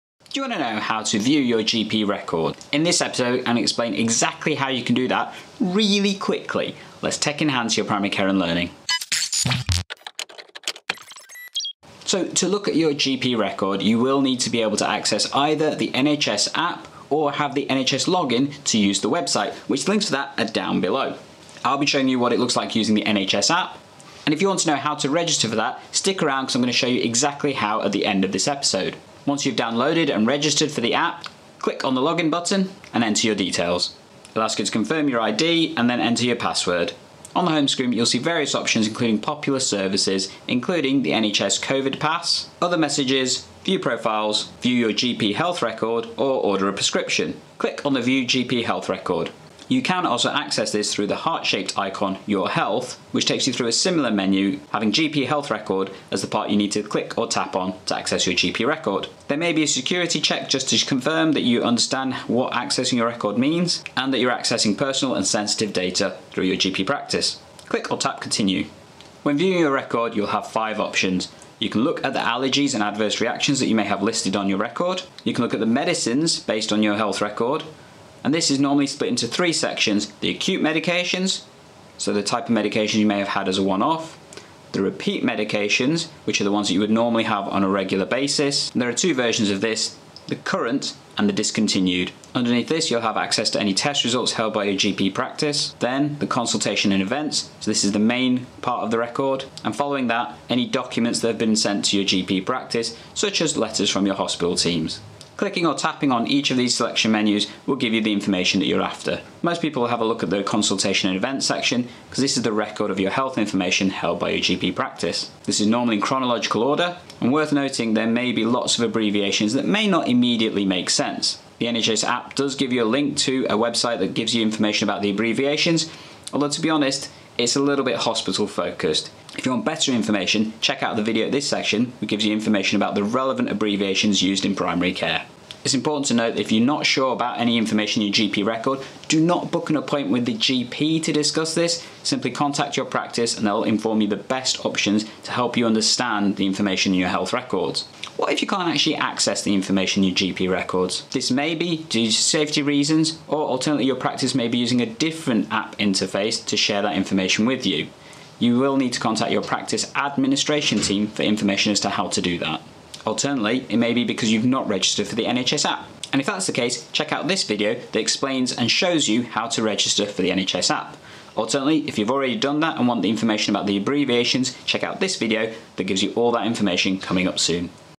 Access your NHS GP medical records
How to access your NHS GP medical records via the NHS app

00:00 Access your records
00:16 NHS app or web login
02:15 I don't understand my records
02:30 What if it doesn't work?